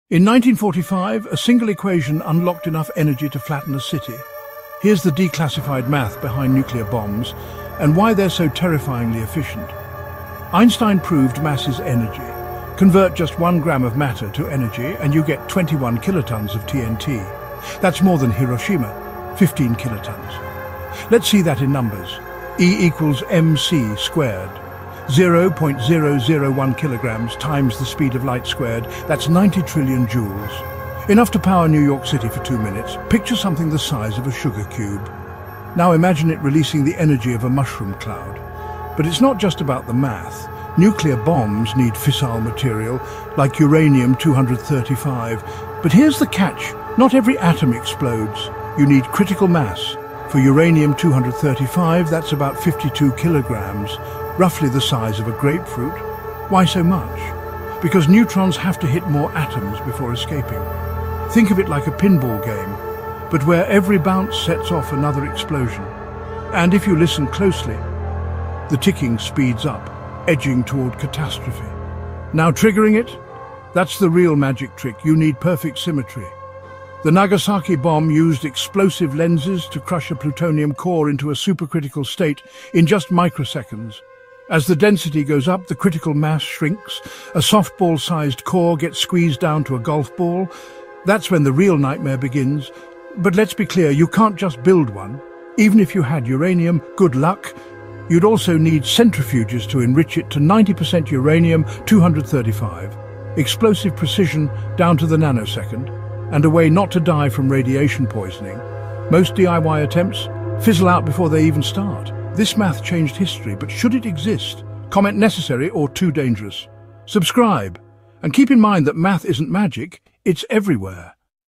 The Terrifying Math Behind Nuclear Bombs
Discover the chilling science behind nuclear bombs in just over 2 minutes. This video breaks down the *declassified physics* and terrifying math that powers atomic explosions, starting with Einstein’s famous equation E=MC² and how converting just 1 gram of matter releases energy equivalent to 21 kilotons of TNT—enough to devastate a city. Learn about the critical mass needed to sustain a chain reaction, the implosion triggers that compress plutonium to supercritical density, and why building a bomb is far from simple. With visually striking animations and an ominous soundtrack, this video unpacks the awe-inspiring yet sobering reality of nuclear weaponry.